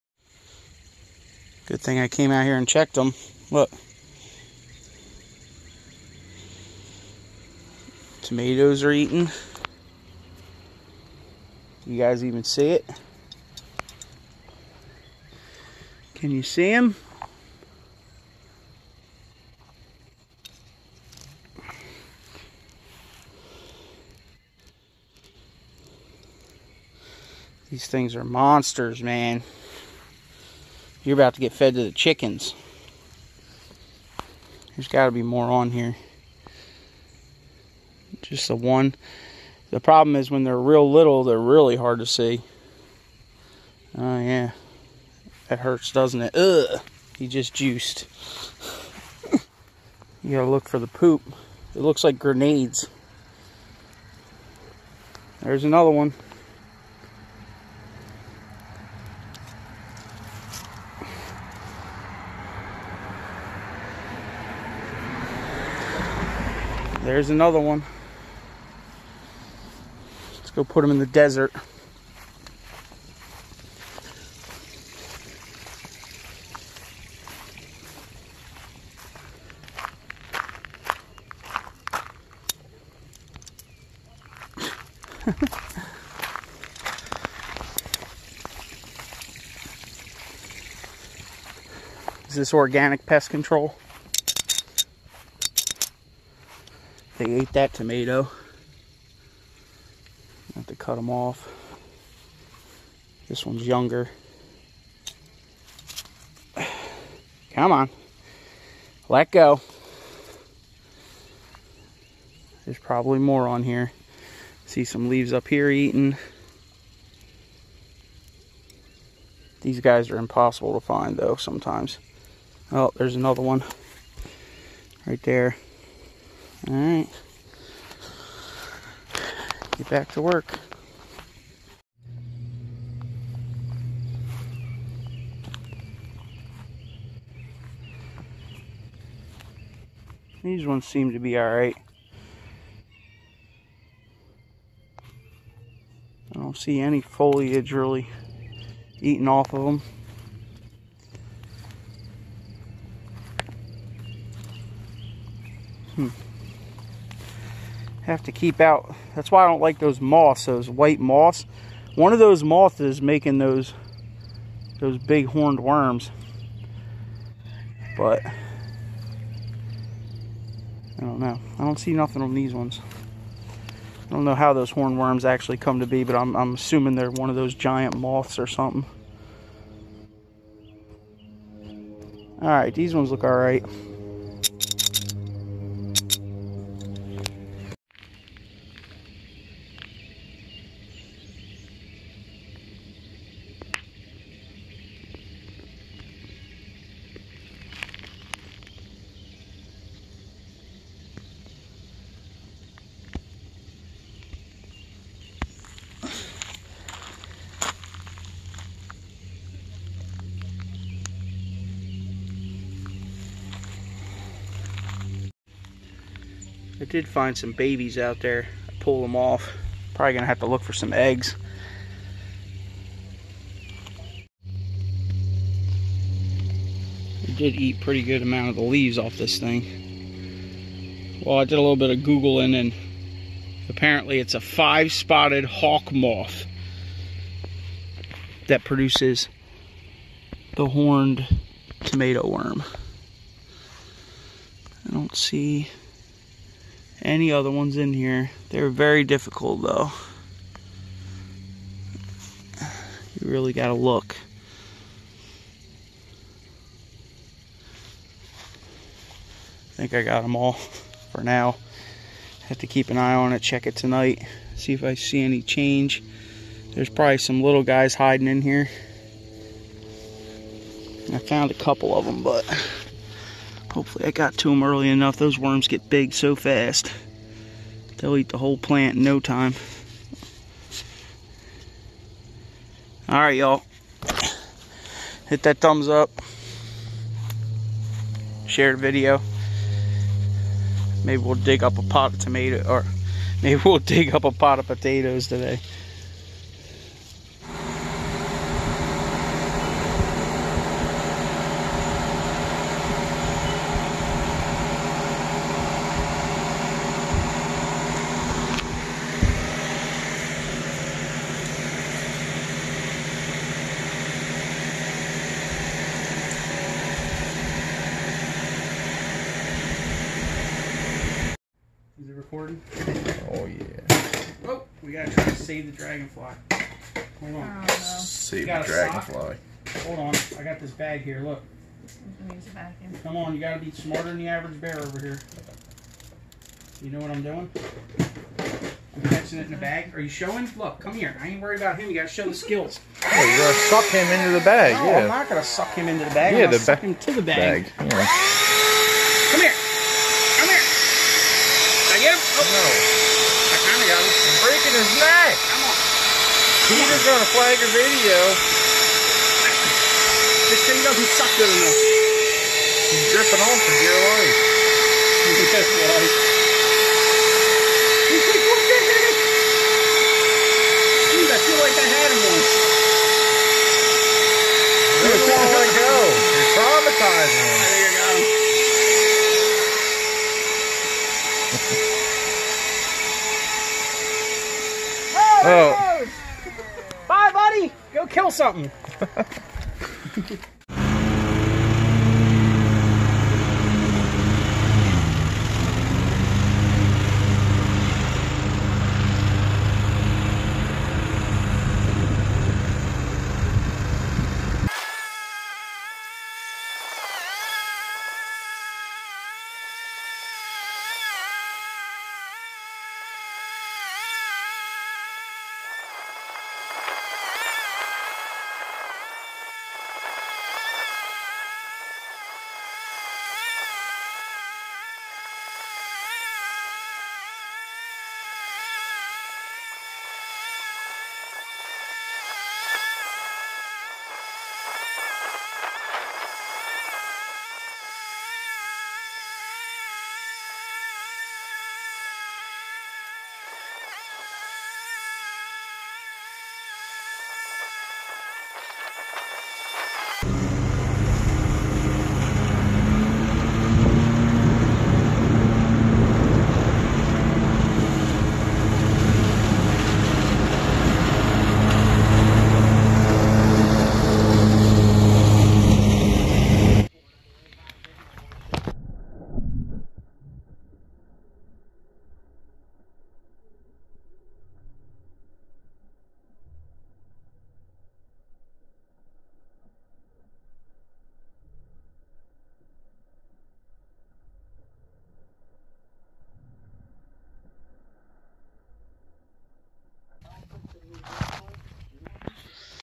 Horned Tomato worms and other shinanigans
a little bit in the garden and some other random stuff.